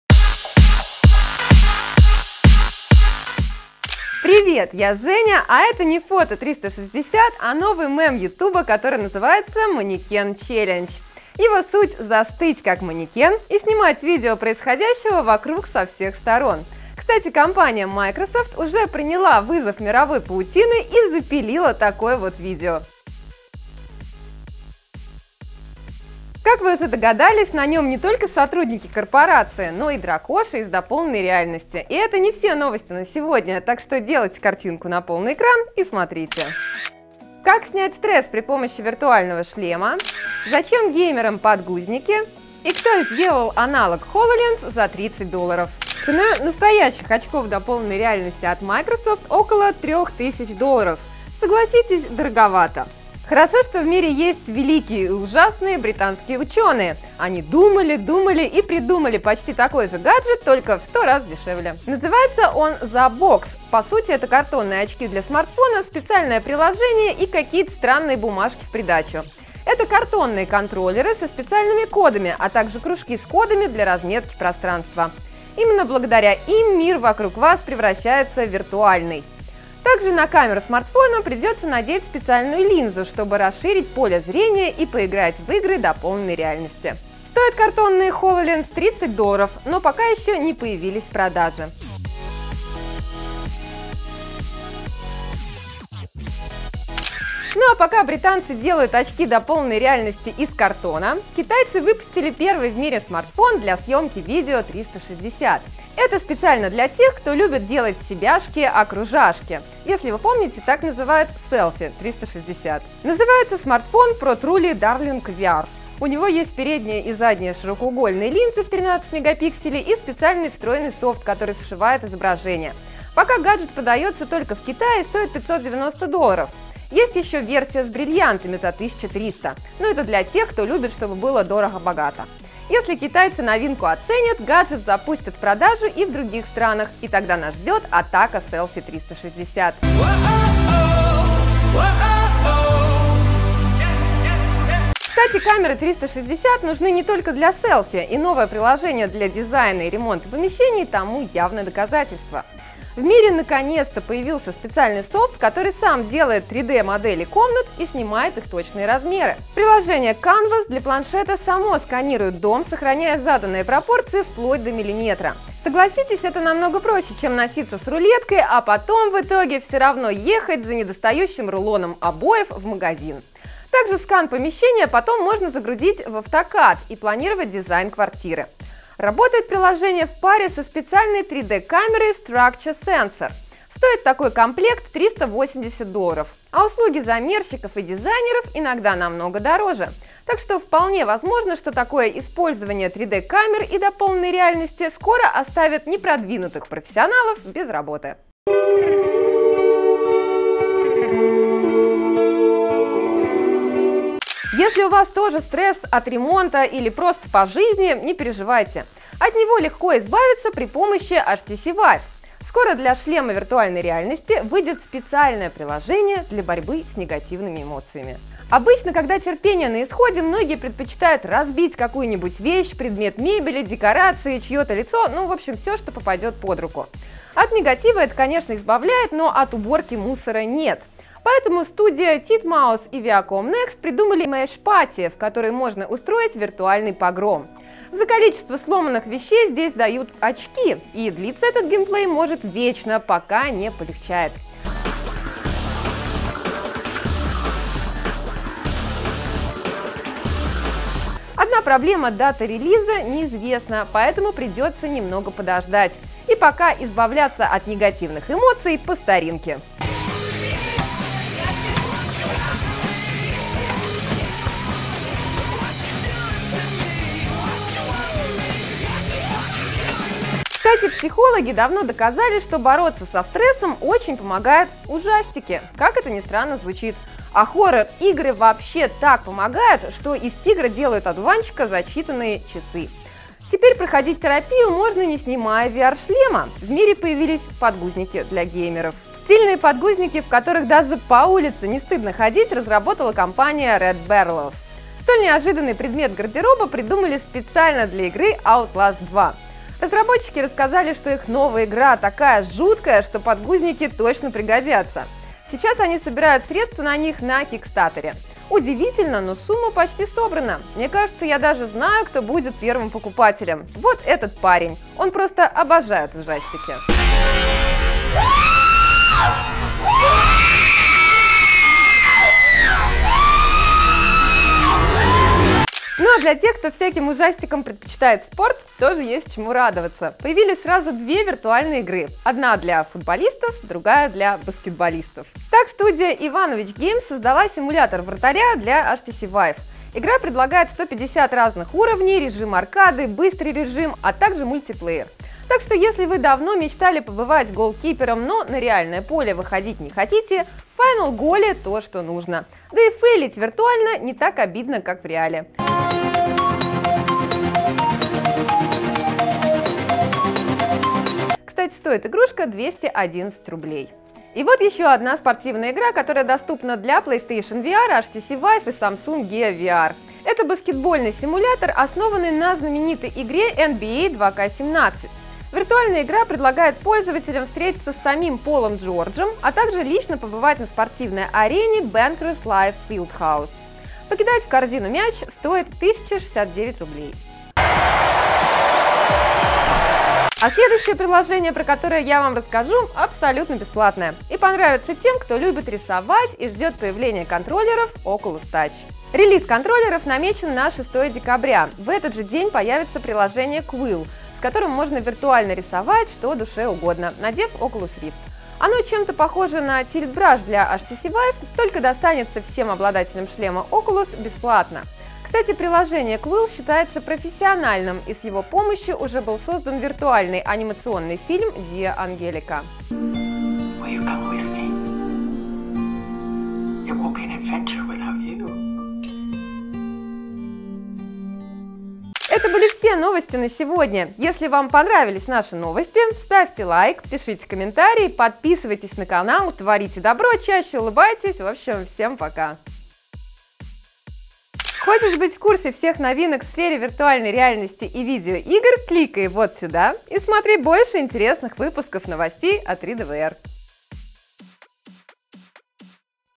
#29 Новости виртуальной реальности: обзор новинок игр и гаджетов.
Хочешь быть в курсе всех новинок в сфере виртуальной реальности и видео игр? Обзор последних достижений современной компьютерной техники и рецензии на самые свежие игры в новостях виртуальной реальности от 3D-VR!

1) ZapBox. Цена настоящих очков виртуальной реальности Microsoft Hololens около $3000. Хорошо, что в мире есть великие и ужасные британские ученые. Они придумали почти такой же гаджет только в 100 раз дешевле.
Называется он ZapBox. По сути, это картонные очки виртуальной реальности для смартфона, специальное приложение и картонные контроллеры со специальными кодами, а также кружки с кодами для разметки пространства. Именно благодаря им мир вокруг вас превращается в виртуальный. Также на камеру смартфона придётся надеть специальную линзу, чтобы расширить поле зрения и поиграть в виртуальные игры и приложения. Стоят картонные Microsoft Hololens 30 долларов, но пока еще не появились в продаже.

2) Protruly Darling VR. Пока британцы делают очки дополненной реальности из картона, китайцы выпустили первый в мире смартфон для съемки видео 360!  Это специально для тех кто любит делать селфи 360.
Называется смартфон Protruly Darling VR. У него есть передняя и задняя широкоугольные линзы в 13 мегапикселей и специальный встроенный софт, который сшивает изображение.  Пока гаджет продается только в Китае и стоит 590 долларов. Если китайцы новинку оценят, гаджет запустят в продажу и в других странах и тогда нас ждет атака селфи 360!
3) Canvas. Кстати, камеры 360 нужны не только для селфи. И новое приложение  для дизайна и ремонта помещений тому явное доказательство. В мире наконец-то появился специальный софт, который сам делает 3d модели комнат и снимает их точные размеры.
Приложение Canvas для планшета само сканирует дом, сохраняя заданные пропорции вплоть до миллиметра. Также скан помещения потом можно загрузить в AutoCAD  и планировать дизайн квартиры. Работает приложение в паре со специальной 3D камерой Structure Sensor. Стоит такой комплект $380. Вполне возможно, что такое использование 3D камер и дополненной реальности скоро оставит непродвинутых профессионалов  без работы.

4) Smash Party. Скоро для шлема виртуальной реальности HTC Vive выйдет специальное приложение для борьбы с негативными эмоциями.
Студии Titmouse и Viacom Next придумали игру Smash Party, в которой можно устроить виртуальный погром. За количество сломанных вещей здесь даже дают очки, и длиться этот геймплей может вечно- пока не полегчает. Дата релиза пока неизвестна, поэтому придется немного подождать.

5) Underscares. Кстати, психологи давно доказали, что бороться со стрессом очень помогают ужастики. Как это ни странно звучит. А хоррор игры вообще так помогают, что из тигра делают одуванчика за считанные часы. Теперь проходить терапию можно, не снимая VR шлема. В мире появились подгузники для геймеров.
Стильные подгузники, в которых даже по улице не стыдно ходить, разработала компания Red Barrles. Столь неожиданный предмет гардероба придумали специально для игры Outlast 2. Разработчики рассказали, что их новая игра такая жуткая, что подгузники точно пригодятся. Сейчас  они собирают средства на них Kickstarter.

6) Final Goalie: Football Simulator. Появились сразу две виртуальные игры. Одна для футболистов, другая для баскетболистов.
Так студия Ivanovich Games создала симулятор вратаря для HTC Vive. VR игра предлагает 150 разных уровней, режим аркады, быстрый режим, а также мультиплеер. Так что, если вы давно мечтали побывать голкипером, но на реальное поле выходить не хотите Final Goalie - то, что нужно. Кстати стоит игрушка 211 рублей.
7) NBA 2KVR Experience. И вот еще одна спортивная VR игра, которая доступна для PlayStation VR, HTC Vive и Samsung Gear VR. Это баскетбольный симулятор, основанный на знаменитой игре NBA 2K17. Виртуальная игра предлагает пользователям встретиться с самим Полом Джорджем, а также лично побывать на спортивной арене Bankers Life Fieldhouse. Покидать в корзину мяч стоит 1069 рублей.
8) Oculus Touch. Quill. Релиз контроллеров Oculus Touch намечен на 6 декабря! В этот же день появится приложение Quill, с которым можно виртуально рисовать, надев очки виртуальной реальности Oculus Rift. Оно чем-то похоже на Tilt Brush для HTC vive тока достанется всем обладателям шлема виртуальной реальности Oculus Rift бесплатно. Кстати, приложение Quill считается профессиональным и с его помощью уже был создан виртуальный анимационный фильм Dear Angelica.